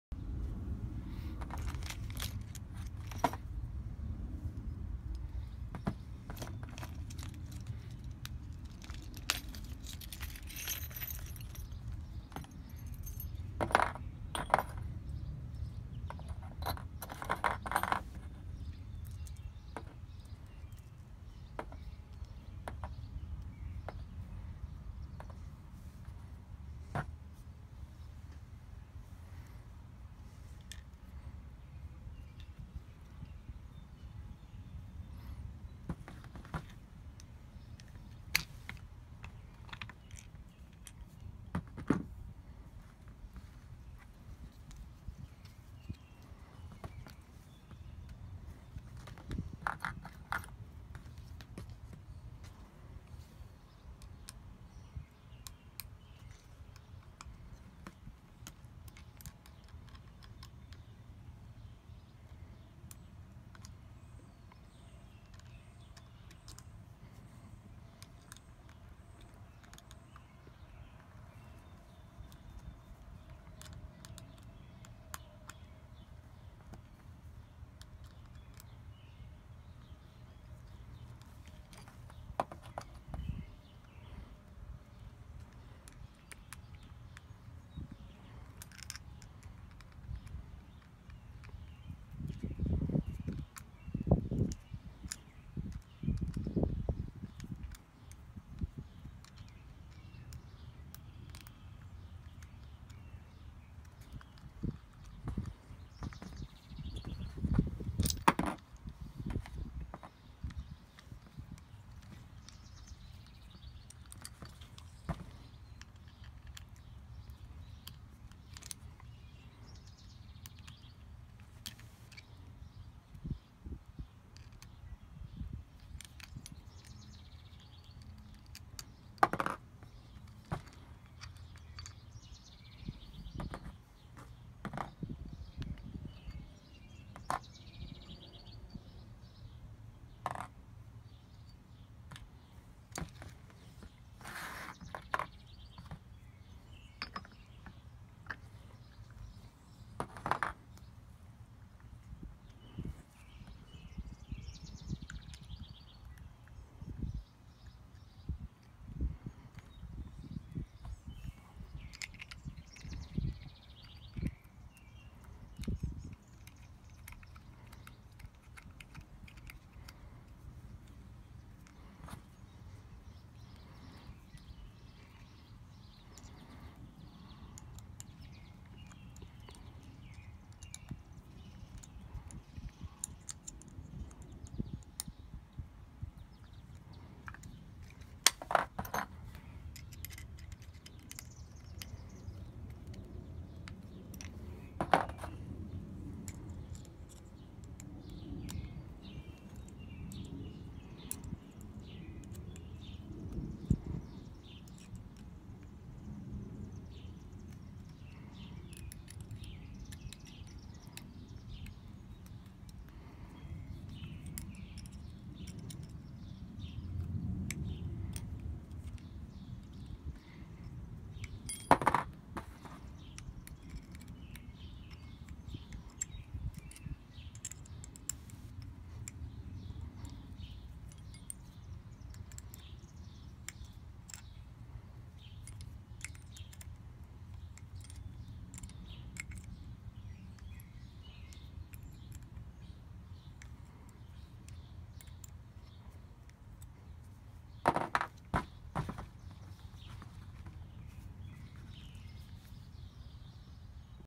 Manfrotto 410 conversion
How to convert Manfrotto 410 into arca type using Hejnar photo conversion